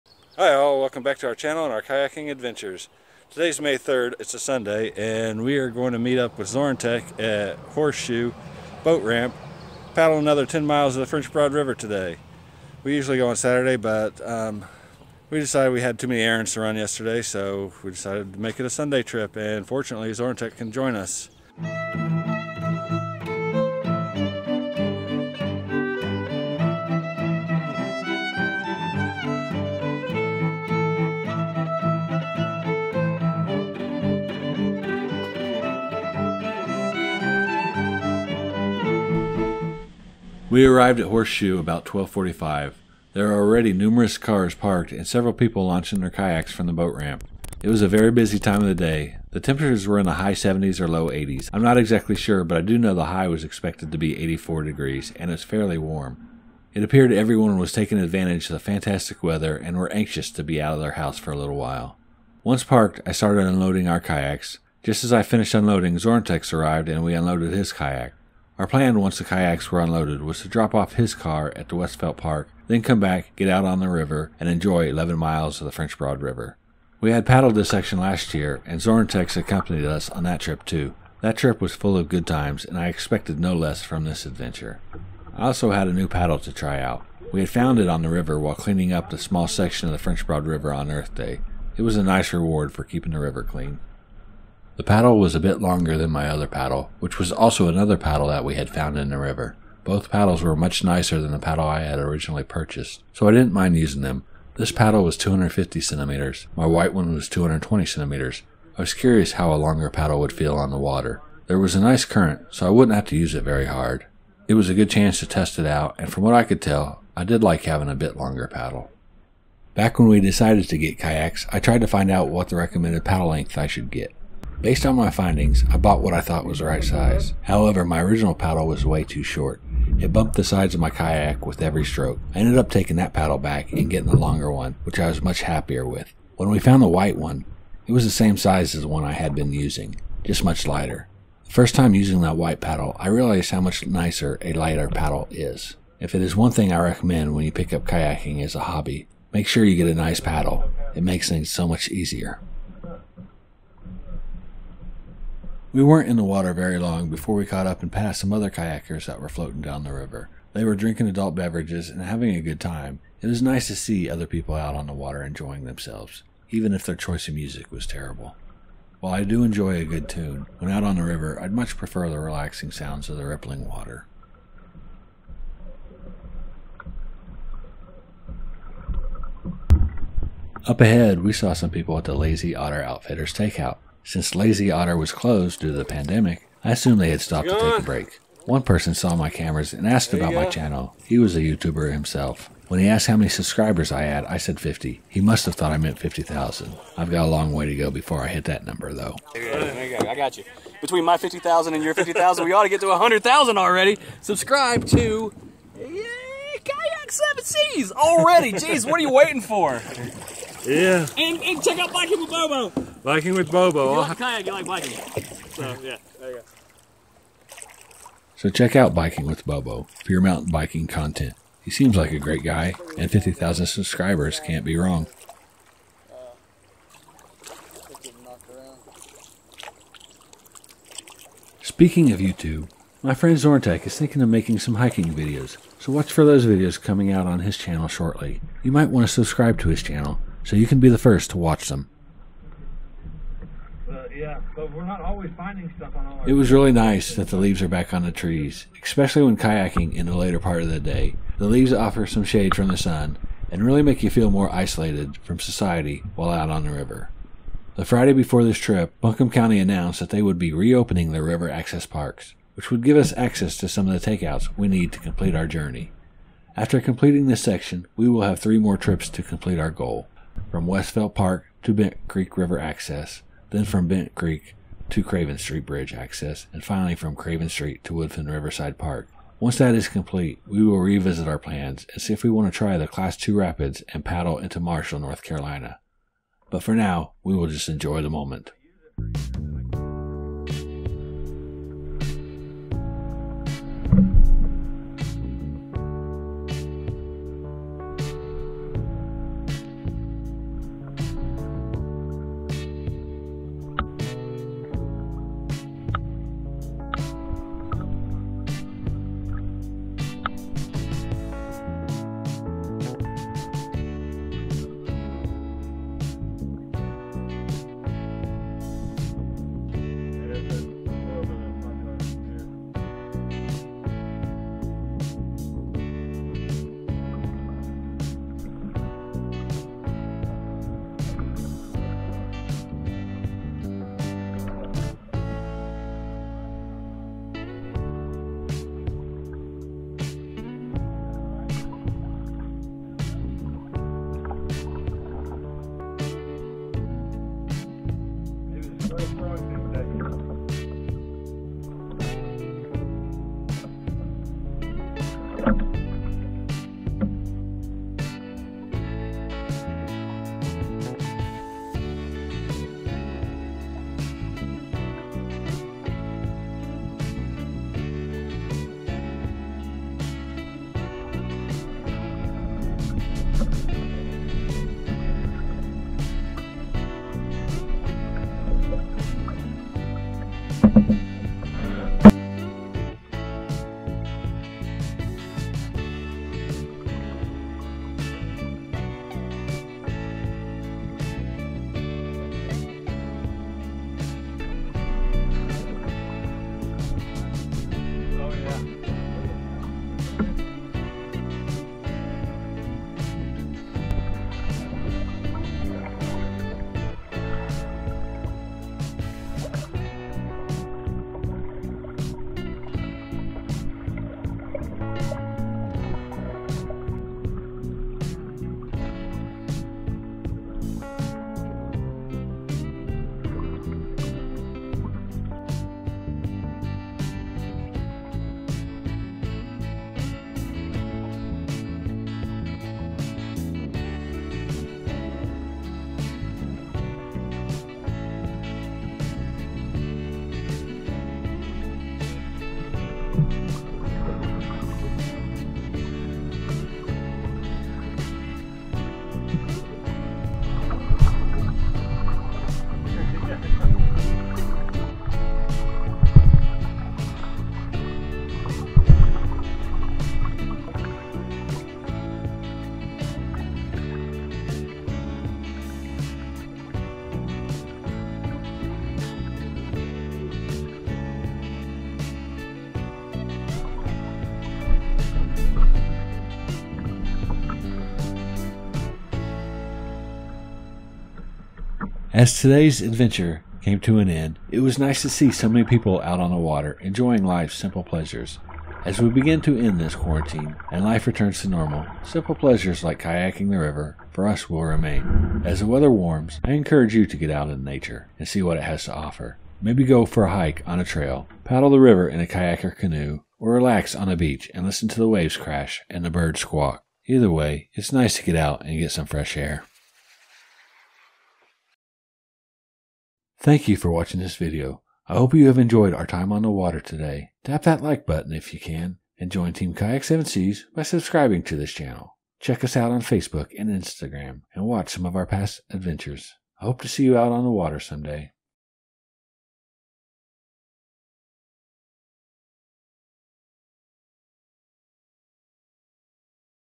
Quarantined: Viva La Kayak Revolution - Horse Shoe Boat Ramp to Westfeldt Park (5-3-20)
As the stay-at-home order continues in North Carolina, we still need some fresh air and exercise.  Kayaking the French Broad River is the best medicine for us to remedy being stuck indoors. We have a goal for 2020 to kayak from the headwater of the French Broad to Woodfin Dam which is ~75 miles. This trip from Horse Shoe Boat access to Westfeldt Park knocks another 11 miles off that goal. Our friend Zorentex was also available to paddle the river with us.